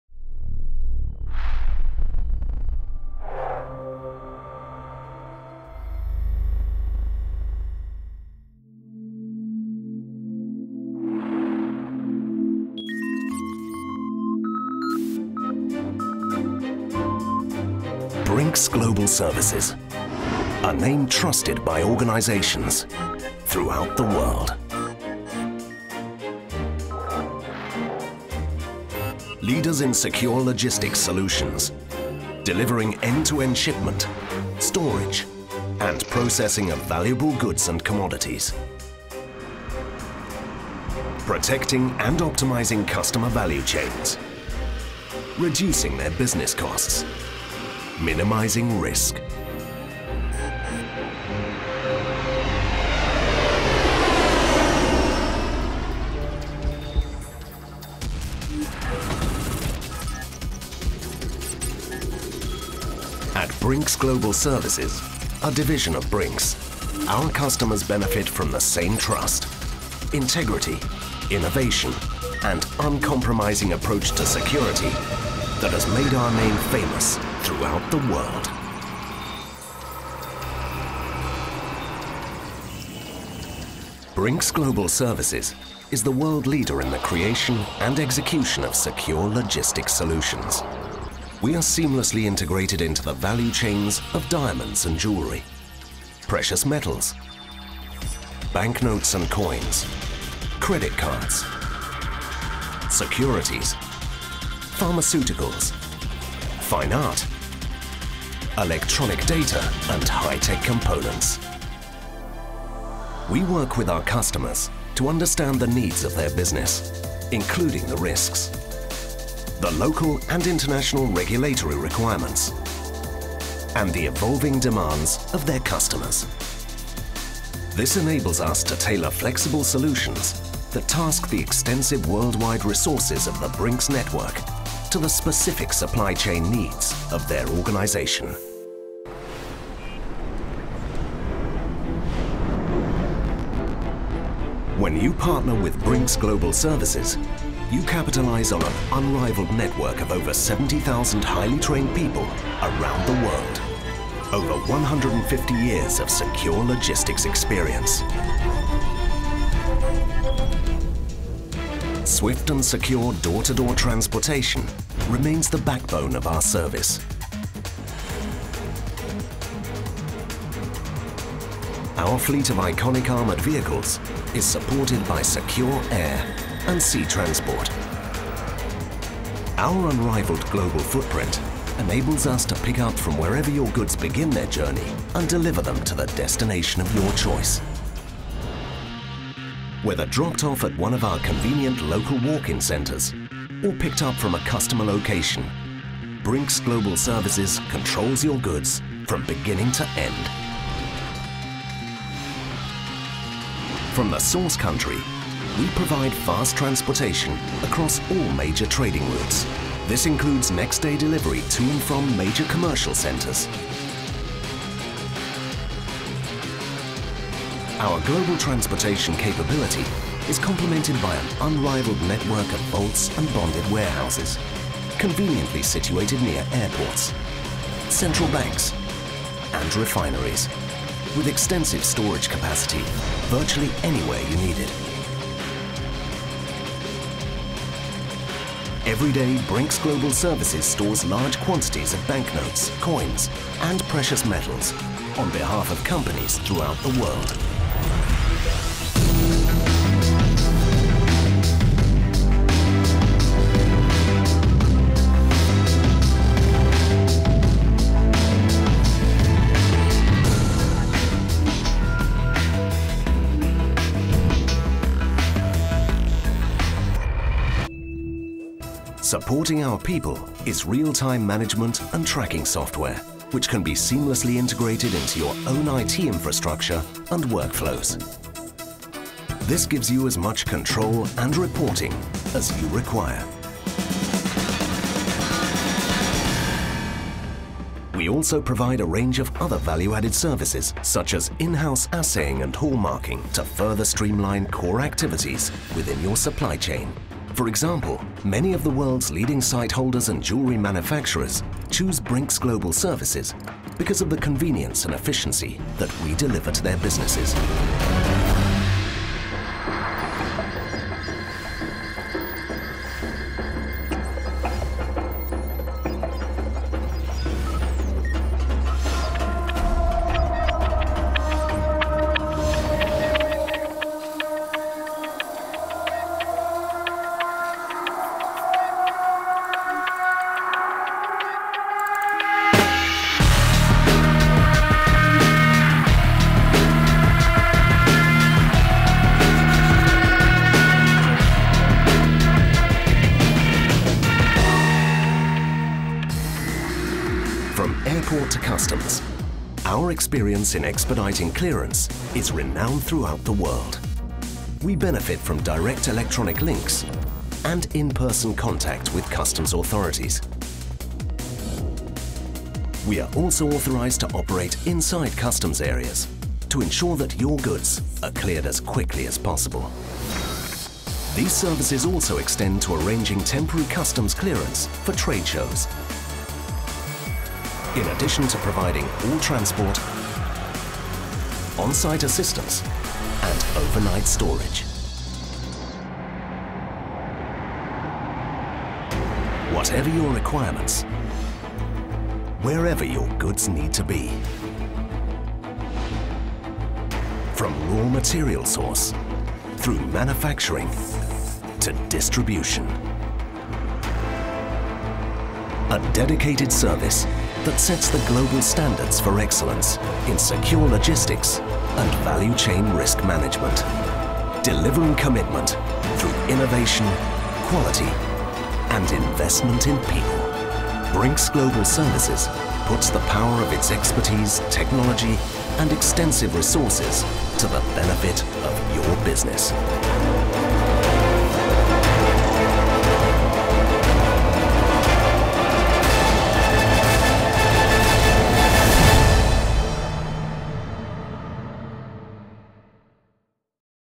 Brink's Global Service | Company Overview
Brink's Global Services: Delivering commitment through innovation, quality and investment in people. See how Brink's global solutions provide end-to-end security around the world.